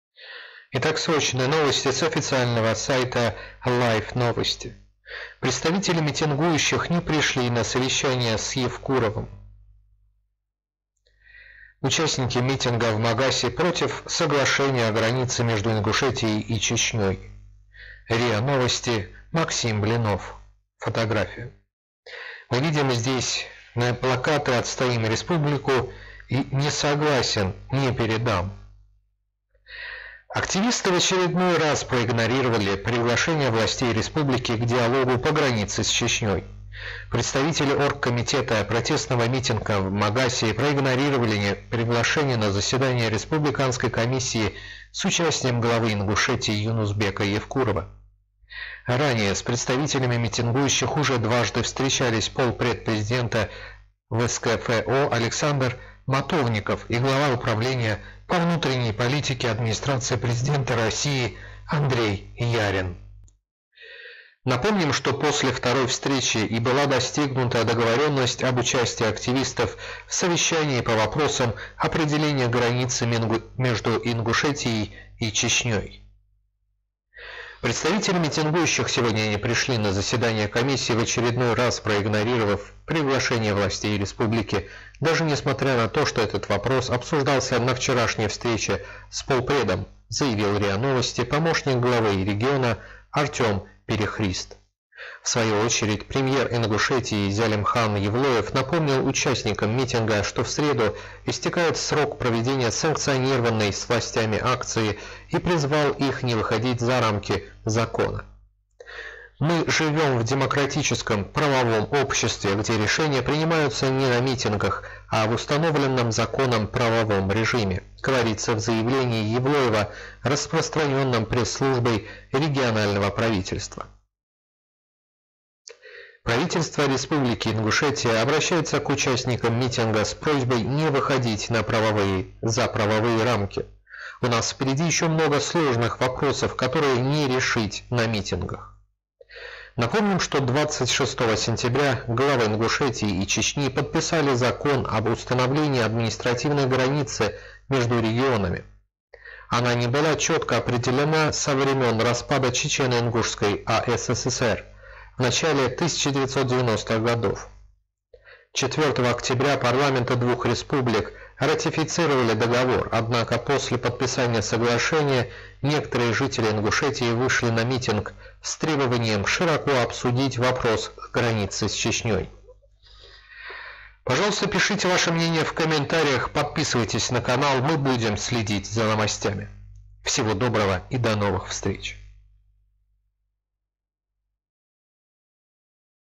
Митингующие не пришли на совещание с Евкуровым в Ингушетии
Активисты в очередной раз проигнорировали приглашение властей республики к диалогу по границе с Чечнёй.